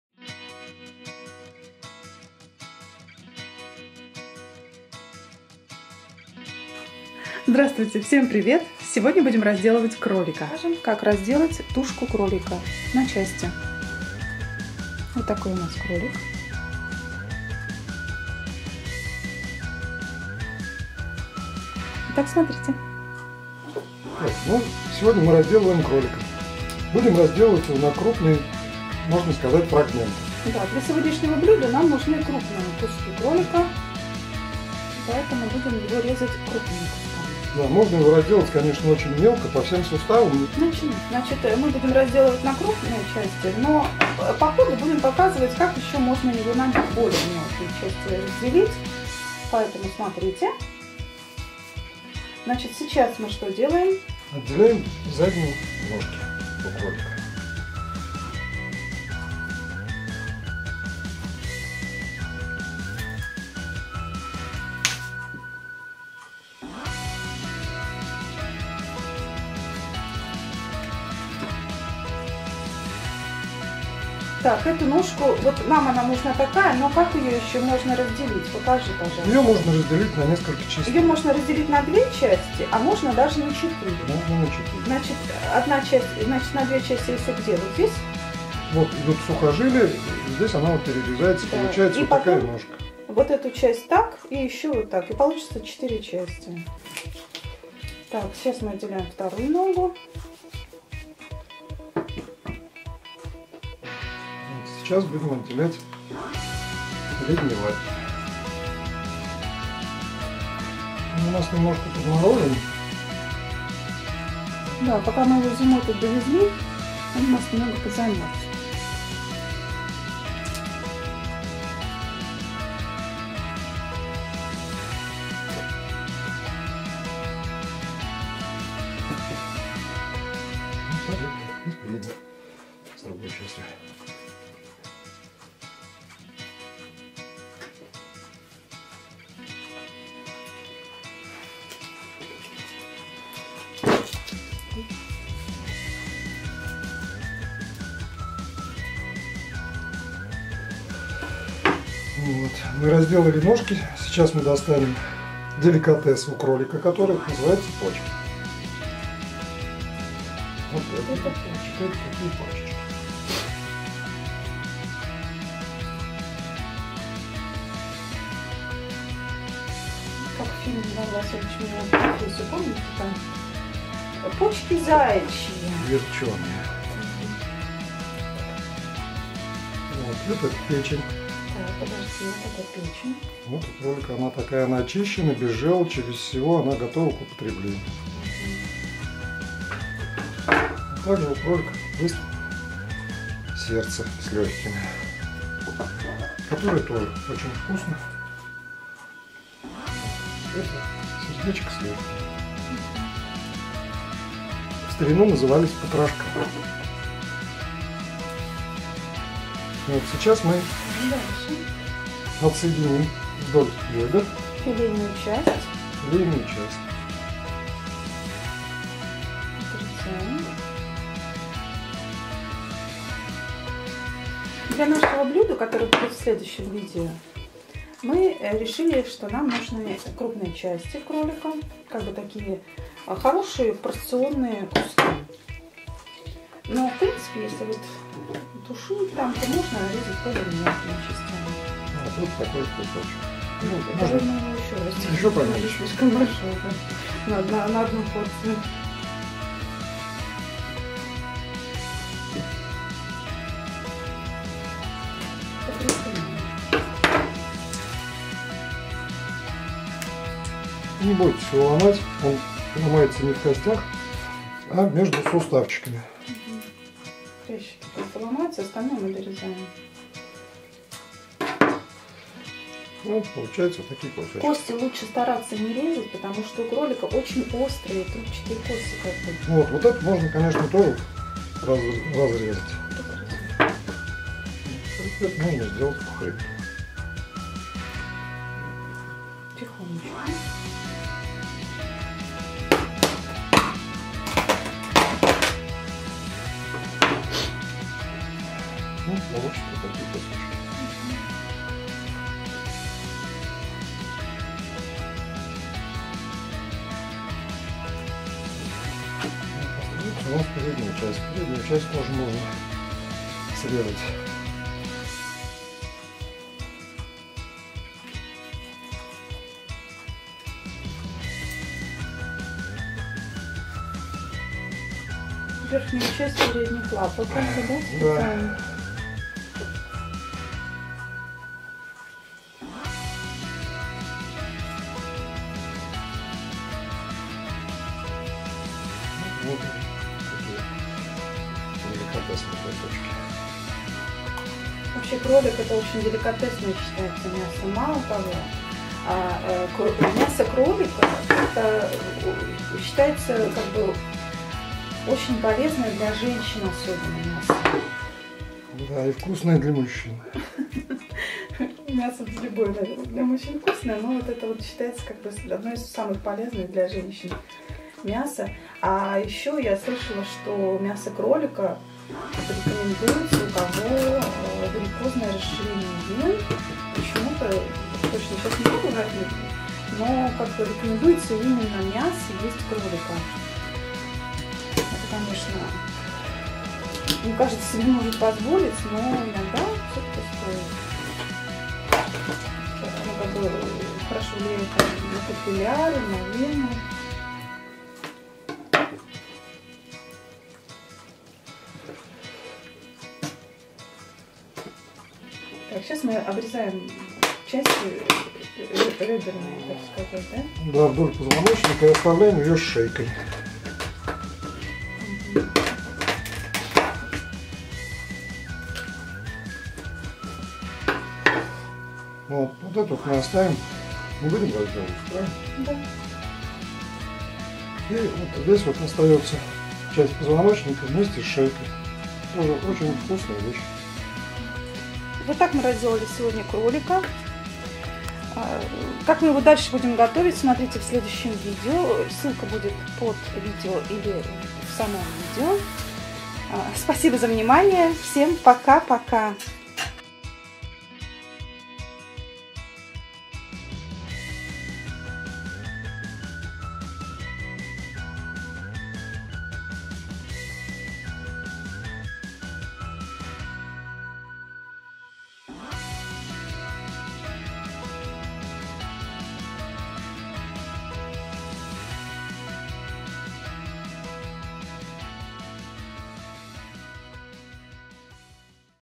Как разделать тушку кролика на части, как разрезать кролика для запекания.
В этом видео я поделюсь с вами секретом, как разделать кролика на части, для приготовления диетического блюда.
Мясо кролика это диетическое мясо, поэтому особо рекомендуется многими диетологами и врачами, для сбалансированного питания.

С уважением.
Лена Геленор (Канал ELENOR2009)